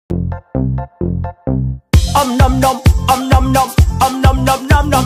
Greenhouse toad finds a cricket.
A toad that was in my greenhouse noms a cricket.